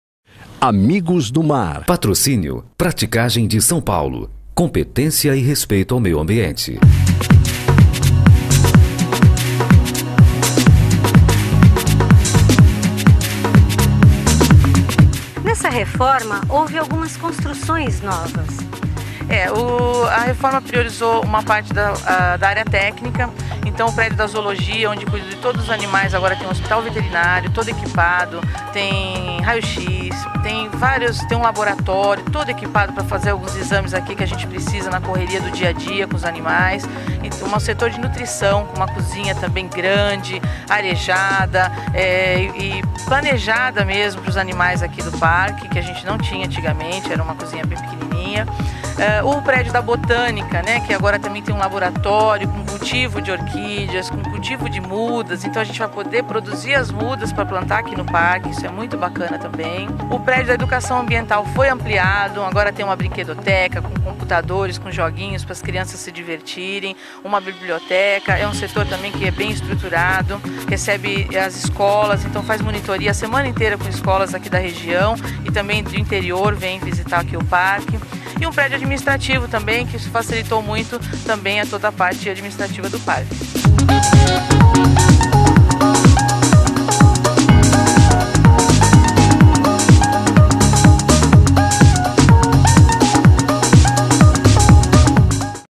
10/2012 - 02 Reforma do Orquidário Municipal de Santos - LIGIA CREMONESI
***Publicado em Outubro de 2012***

Novidades é que não vão faltar no Orquidário Municipal, parque zoobotânico totalmente remodelado, renovado e recheado de novas atrações, a ser entregue oficialmente pela Prefeitura na terça-feira, às10h30, data em que se comemora o Dia Mundial do Meio Ambiente.